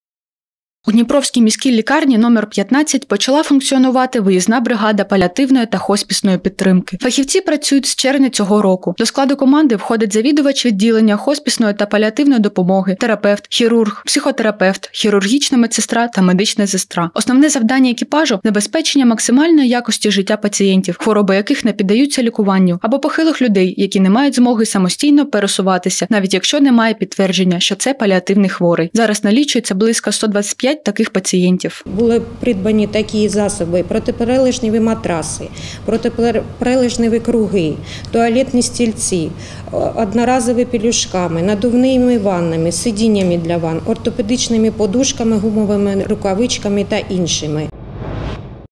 Почала роботу виїзна бригада паліативної допомоги
У Дніпровській міській лікарні №15 почала функціонувати виїзна бригада паліативної та хоспісної підтримки. Фахівці працюють з червня цього року.